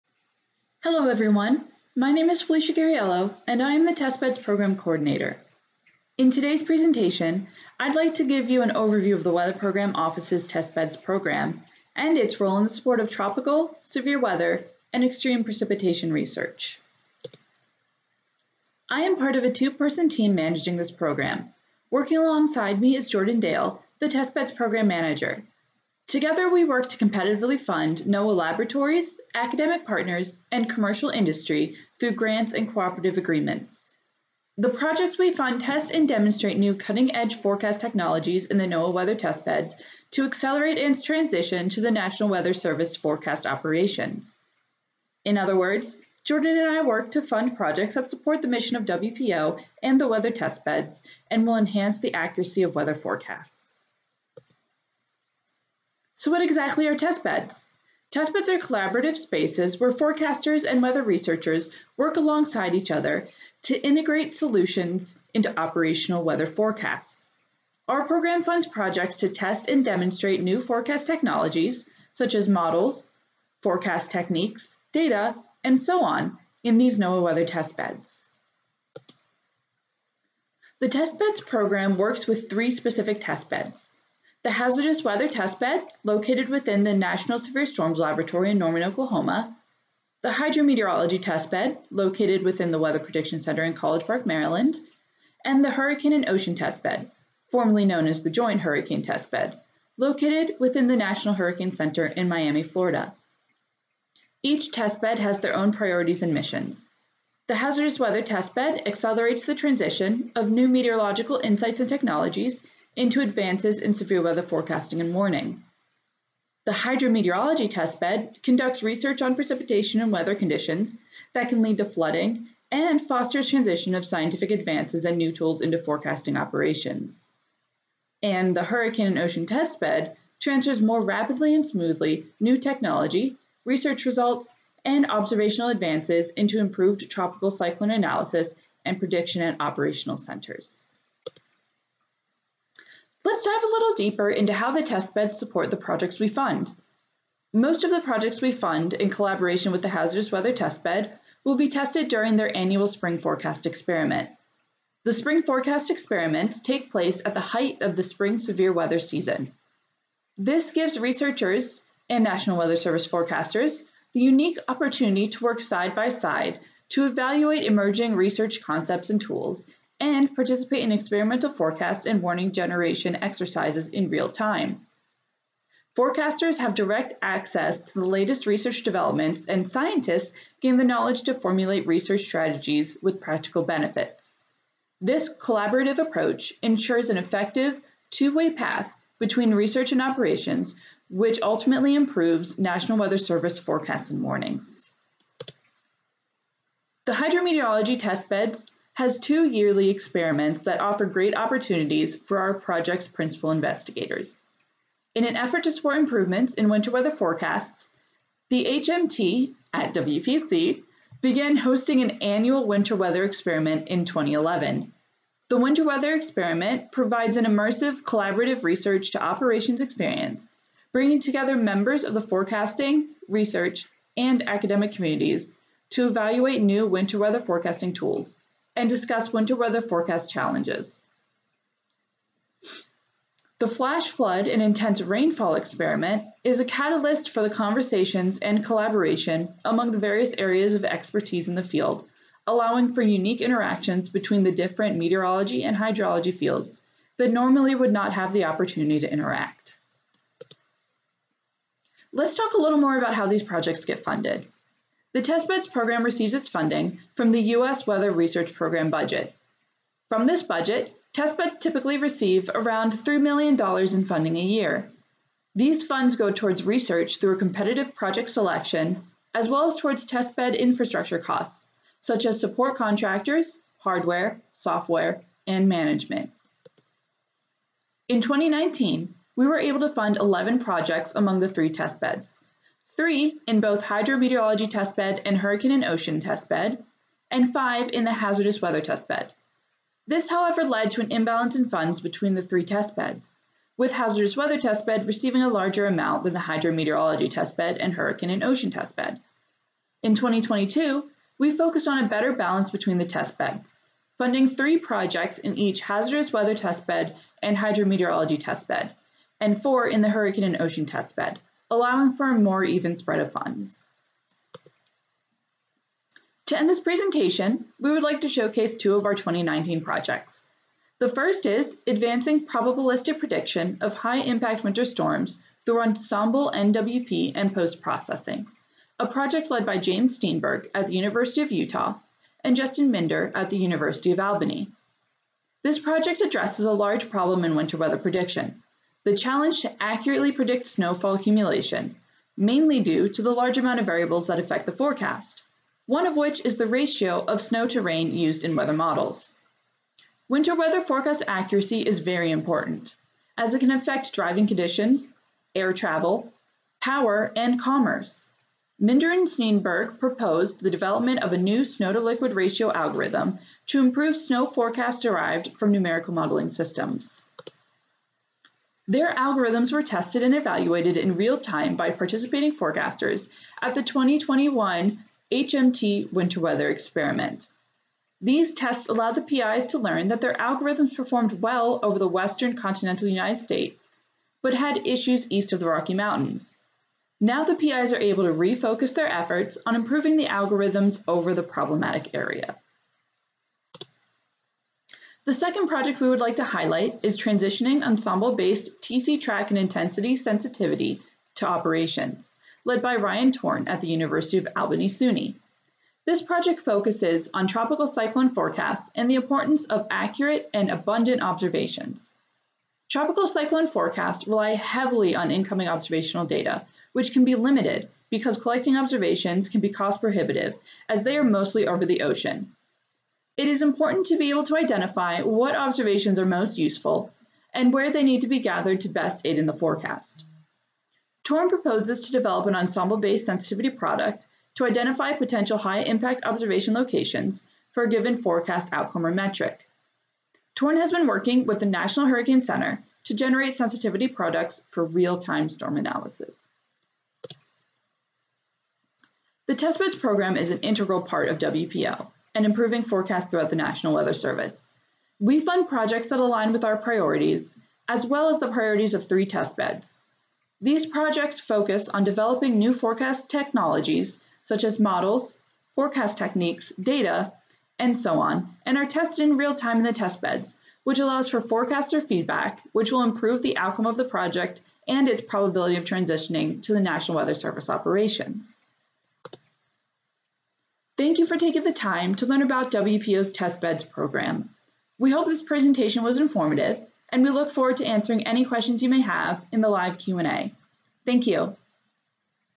Weather Testbeds Program
This video provides an overview of NOAA Research's Weather Program Office Testbeds Program, given by Program Coordinator, Felicia Guarriello. This video was recorded for the Weather Program Office's 5-year Program Review in January 2023.